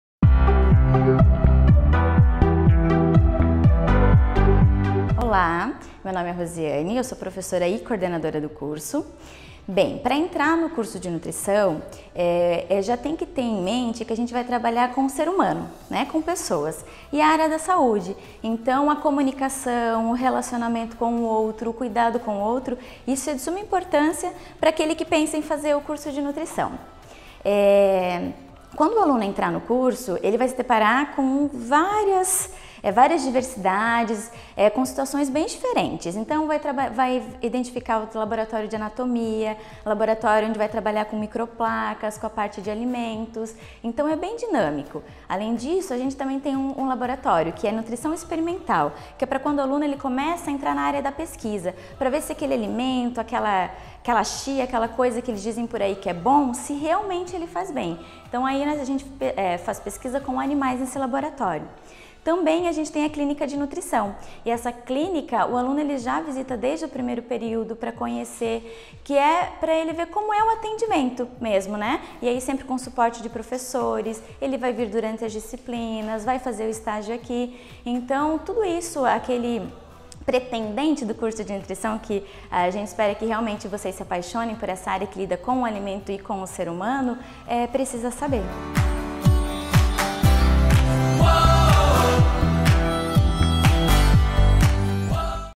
QUERO ESTUDAR – Nutrição Univali / Coordenadora
Laboratórios, clinicas e atendimentos proporcionam a vivência da profissão de Nutricionista.

Acesse queroestudar.univali.br para conhecer mais o Curso de Nutrição.